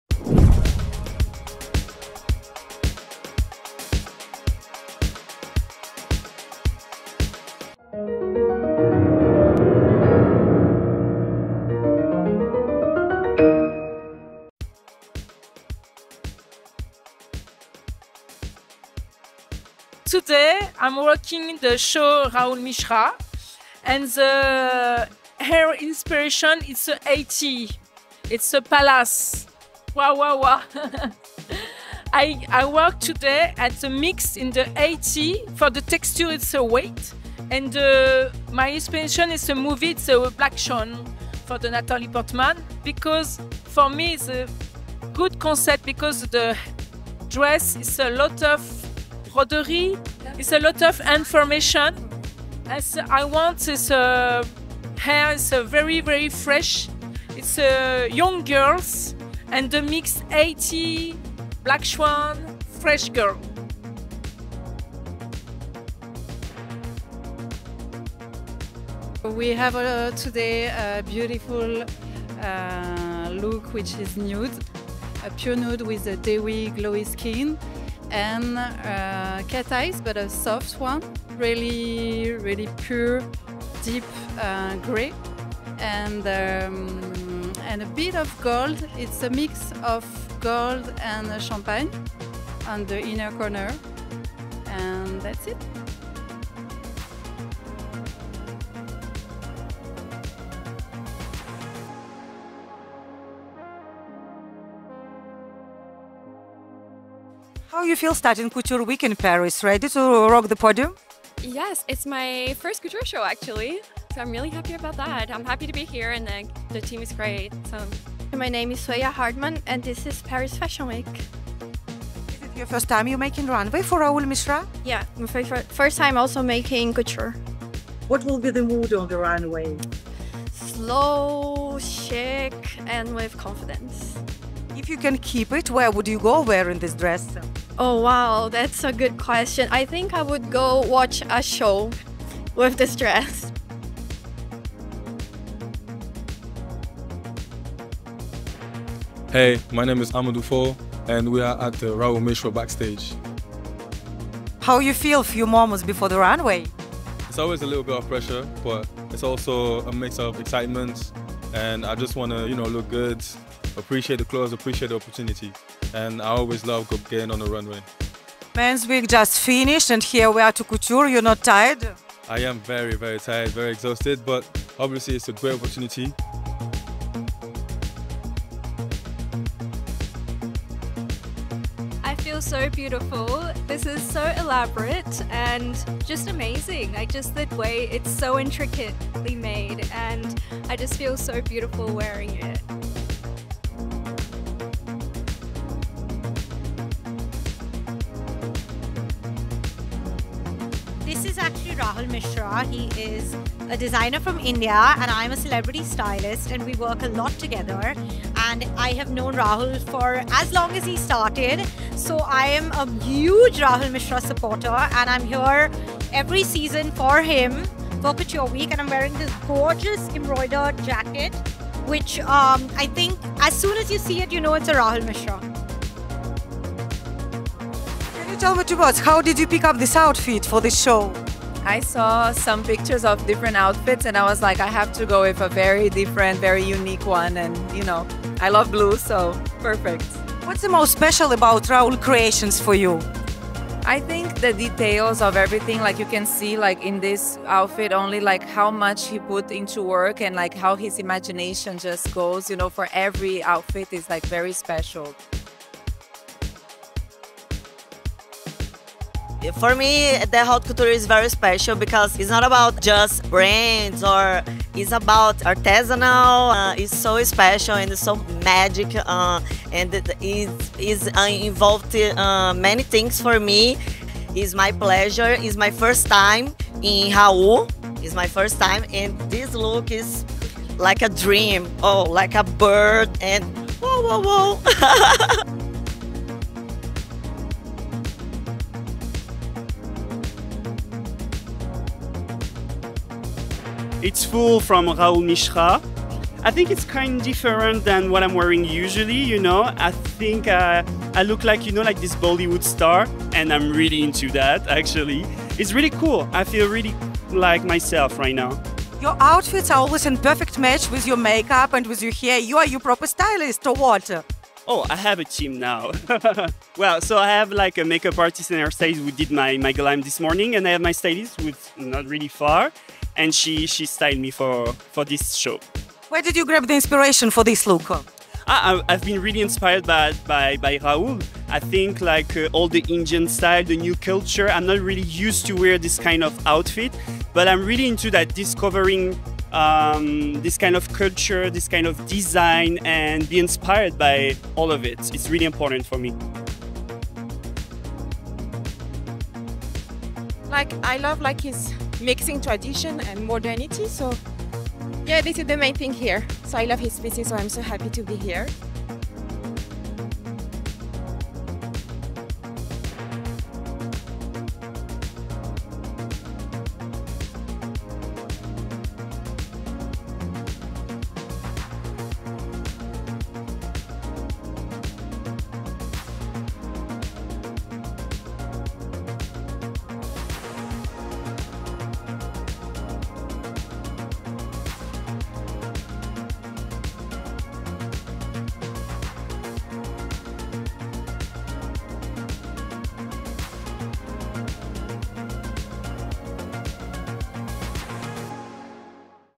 RAHUL MISHRA, Superheroes Haute Couture SS 2024
The "Superheroes" collection by Rahul Mishra highlights the importance of coexisting with reptiles and insects. It emphasizes the impact of human activities on their habitats and the potential threat of extinction to these species.

The collection showcases the essential role of insects in the food chain and pollination process, as well as the support provided by the plant kingdom. It also celebrates the beauty and significance of beetles, bees, flies, snakes, and lizards in nature.

Through couture sculptures, the collection aims to remind us of the crucial role these creatures have played in maintaining the balance of life on our planet for millions of years.